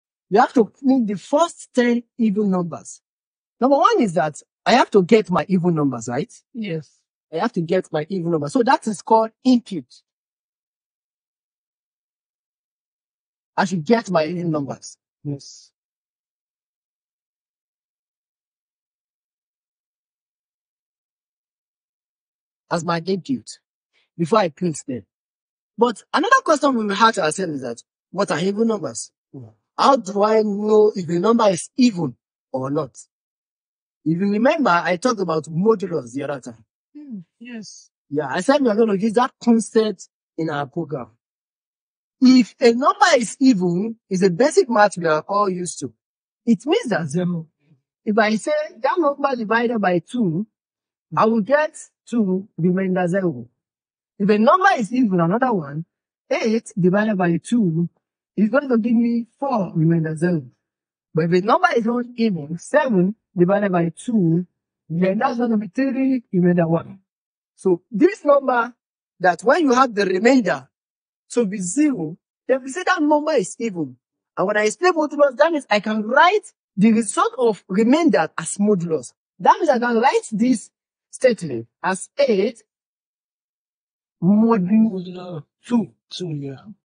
Python for Machine Learning: Even Numbers
How do we even know if a number is even or not? Is there any mathematical explanantion that can help us with that? This video explains exactly that, check it out!!!